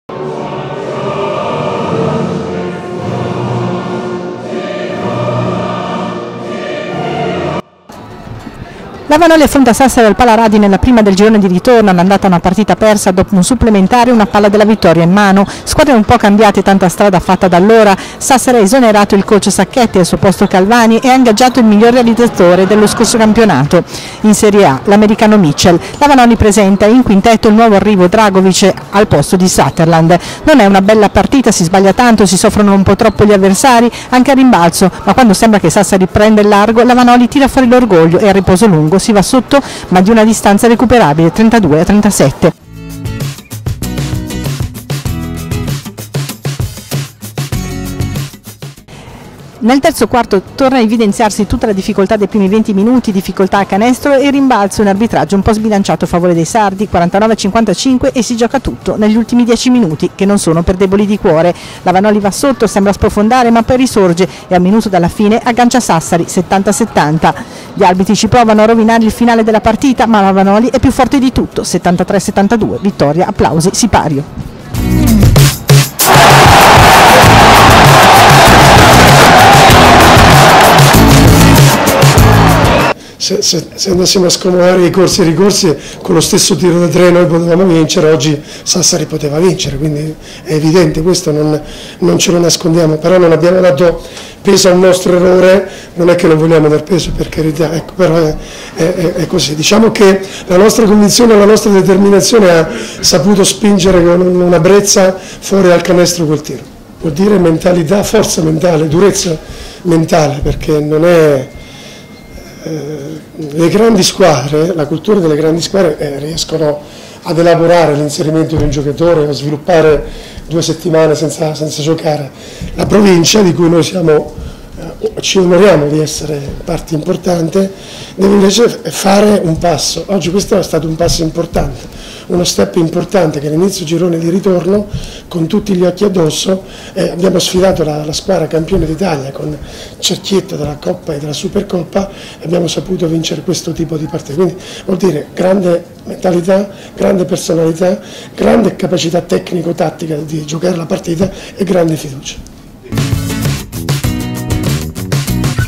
Vanoli - Sassari 73-72. Vittoria al cardiopalmo
Una partita a tratti brutta, ma in cui la Vanoli non ha mai mollato. Sassari è battuta. E i biancoblù continuano a volare.